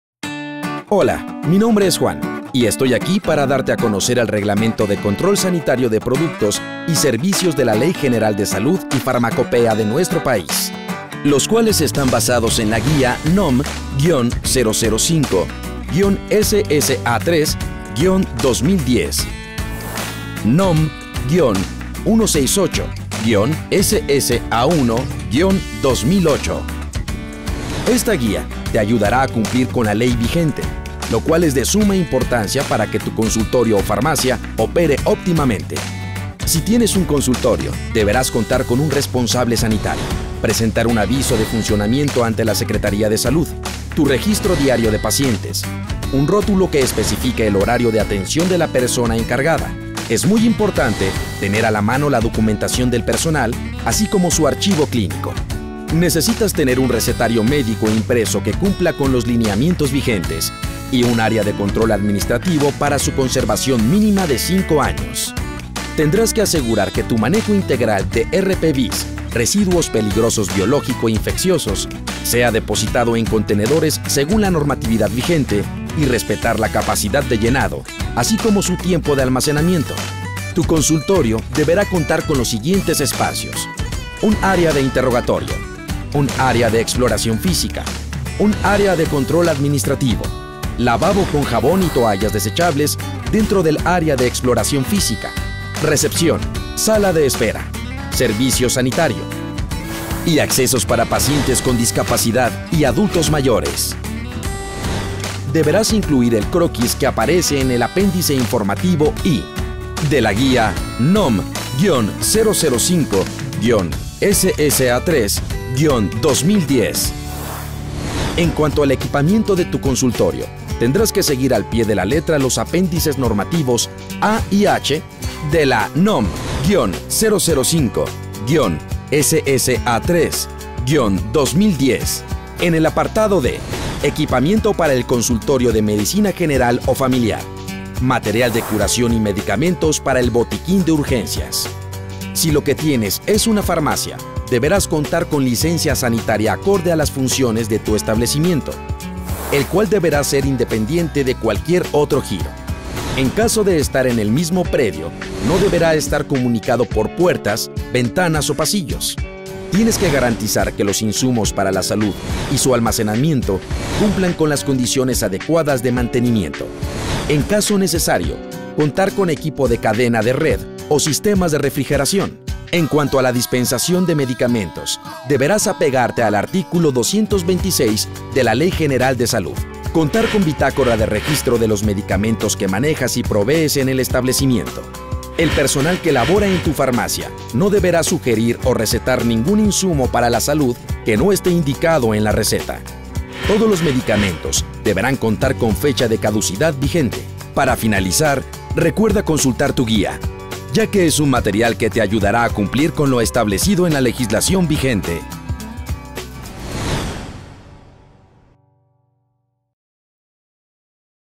Guía para el fomento para las buenas prácticas sanitarias en farmacias y consultorios.
COFEPRIS presenta el reglamento de control sanitario de productos y servicios de la Ley General de Salud y Farmacopea de nuestro país, basados en la guía NOM-005-SSA3-2010, NOM-168-SSA1-2008.

Esta guía ayuda a cumplir con la Ley vigente para que los consultorios o farmacias operen óptimamente.